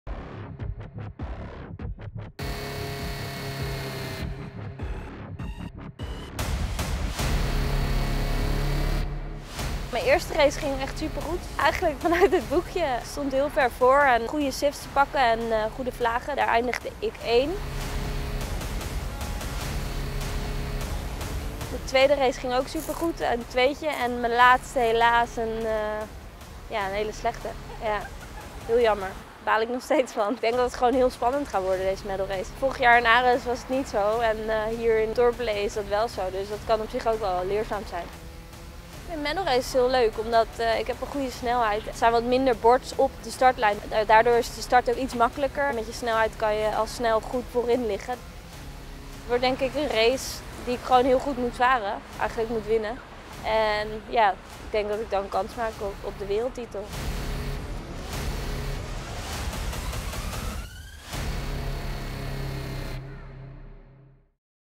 Lilian de Geus drops to 2nd place at RS:X Worlds
After 2 amazing races at the RS:X Worlds, Lilian de Geus was ready to make it a ‘day perfect’. She had sailed a bullet and a 2nd spot. But, due to a bad 3rd race (23rd spot) she has lost the overall lead to Yunxiu Lu.

Saturday the Worlds will end with the concluding medal race (double points). It’s going to be super tight. In order to claim the title, Lilian needs to win the medal race and Yunxiu must finish 4th or higher.